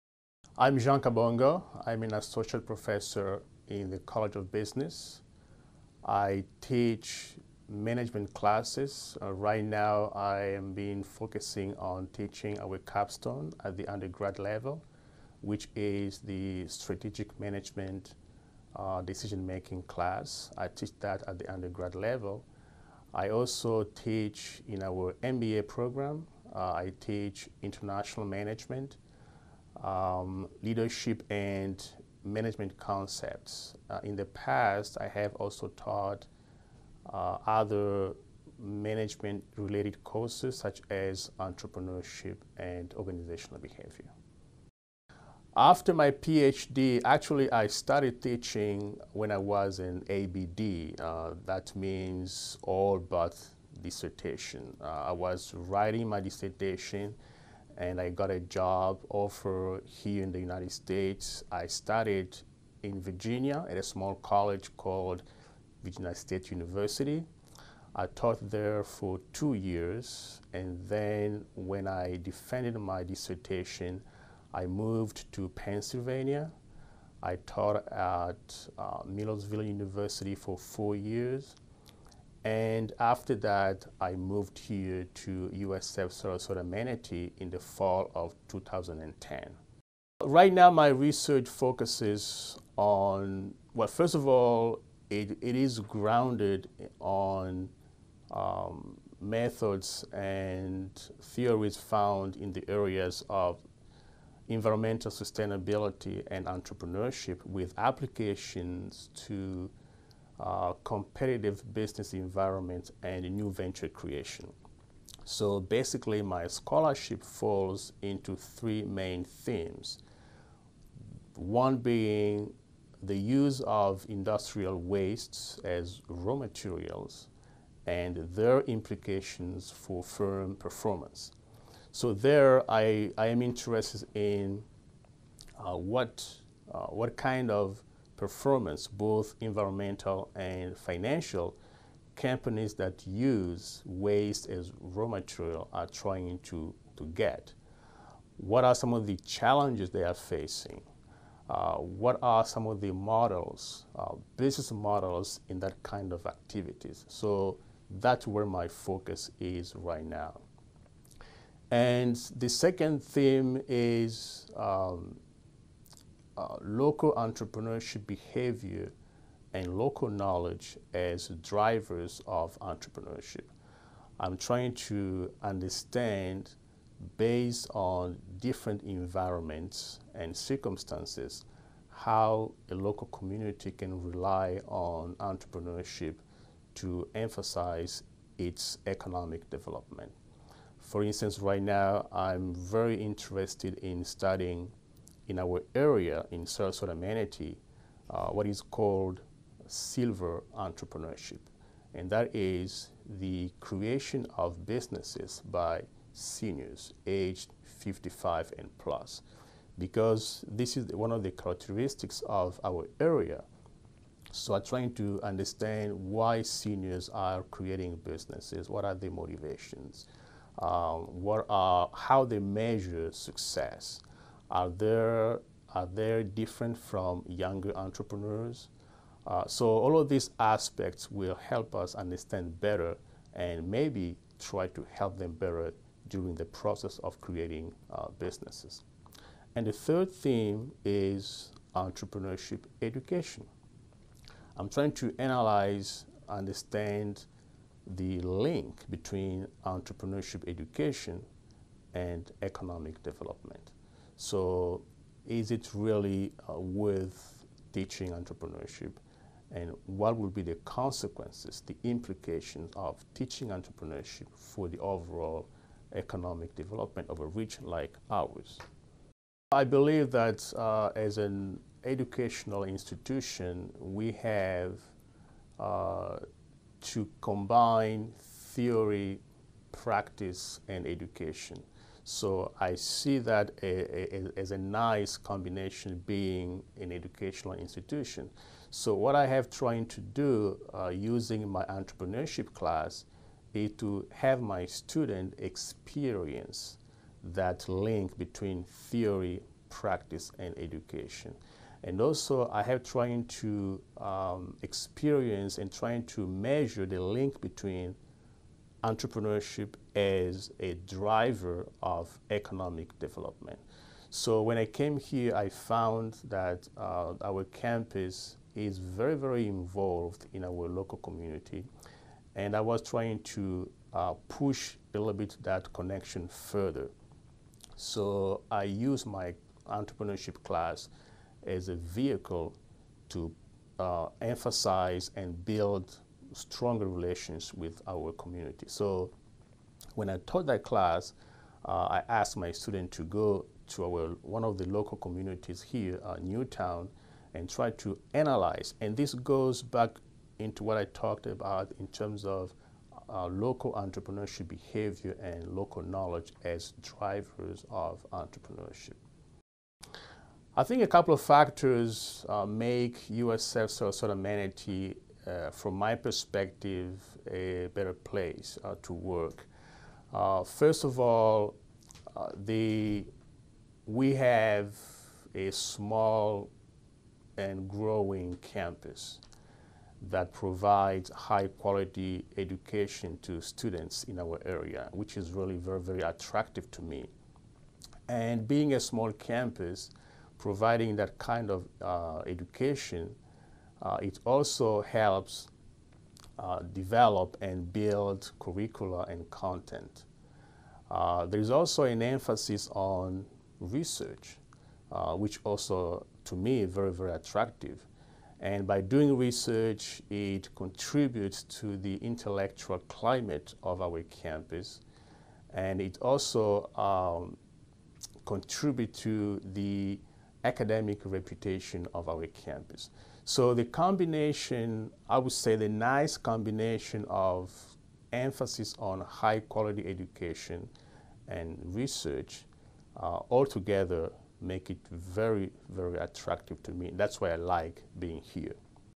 Tuesday Talks: Dr. Jean Kabongo, Strategic Management & Entrepreneurship
Each Tuesday, we sit down with members of the faculty to discuss their expertise, recent research and some of their accomplishments.  This week, USFSM takes a closer look at Dr. Jean Kabongo